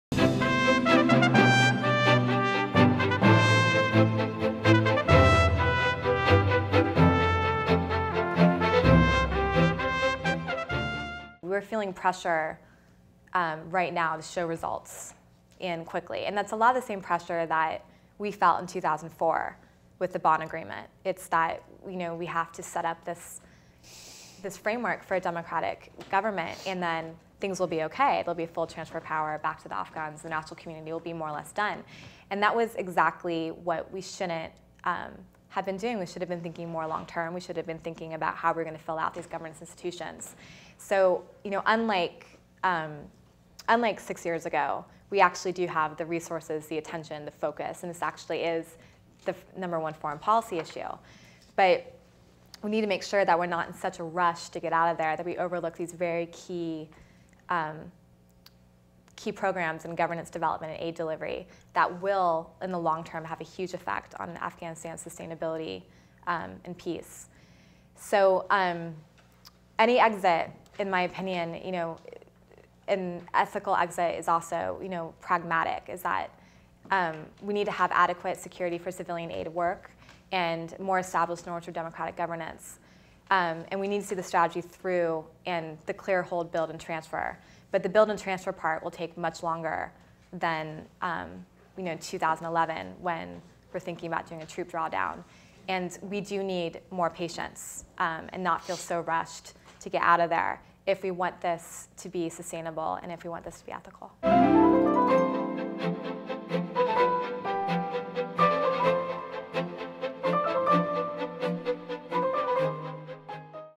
Katherine Brown: Exit from Afghanistan
Katherine Brown states that the United States exit from Afghanistan should not be rushed if we want long-term sustainable development and peace in the country.

This presentation took place at the Carnegie Council on March 16, 2010. For the full video, audio, and transcript, go